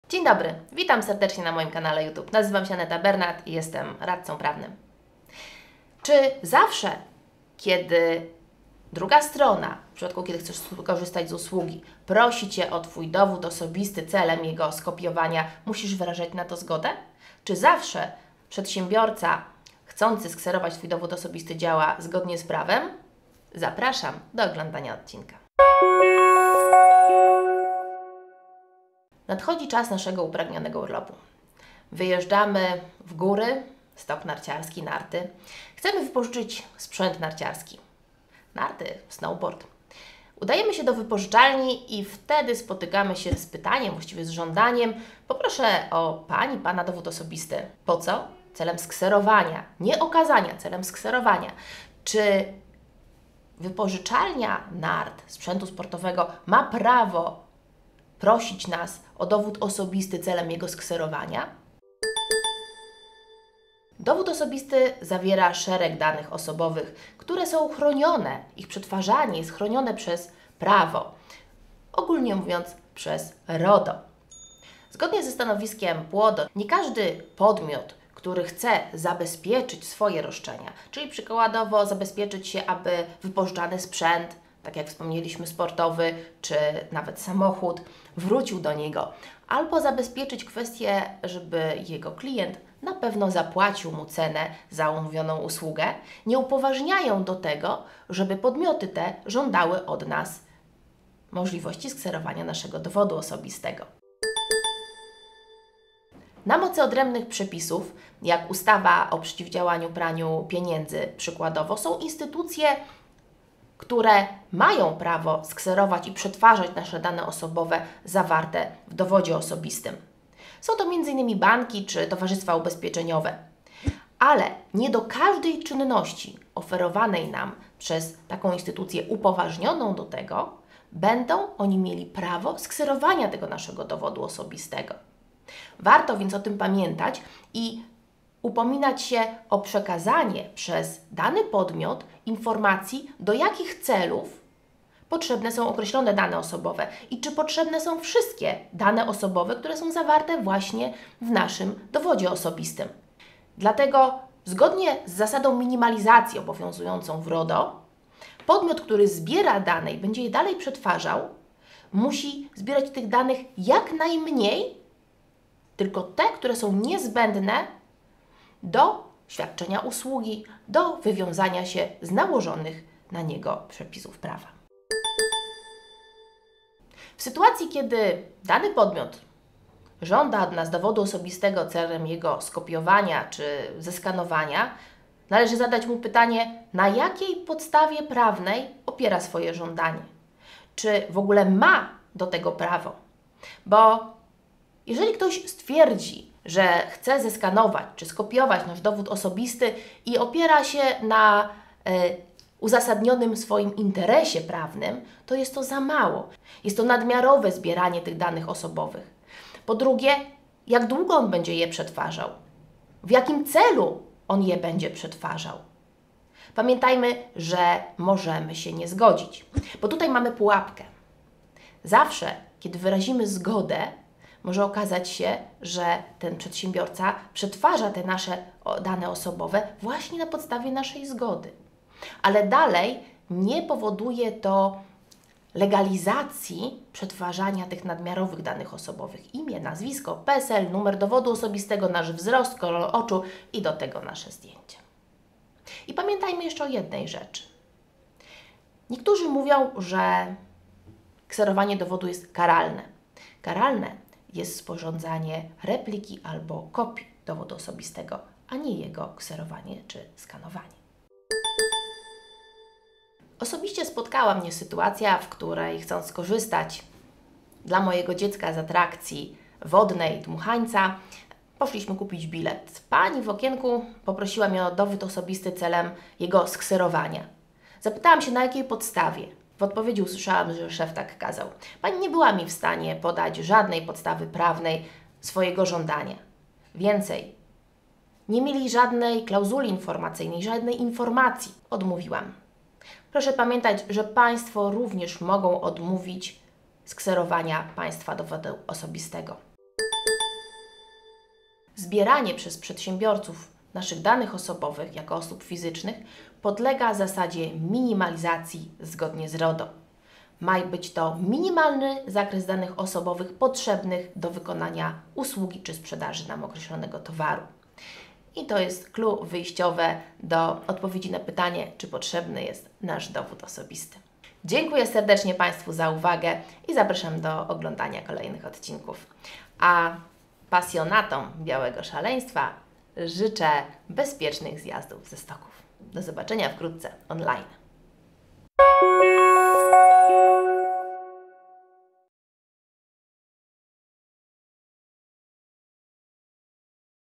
Dane osobowe - kserowanie dowodu osobistego
W tym video dowiesz się:

1:03 Chronione dane w dowodzie osobistym

1:45 Kserowanie dowodu, kto może?

2:55 Jak chronić dane osobowe?

4:22 (nie)Zgoda na kopiowanie danych  osobowych

Zapraszam do wspólnej interpretacji 🤝

Komunikacja
Linki do fb, ig, yt kanał, strona www, blogi (znajdziesz we wcześniejszych opisach)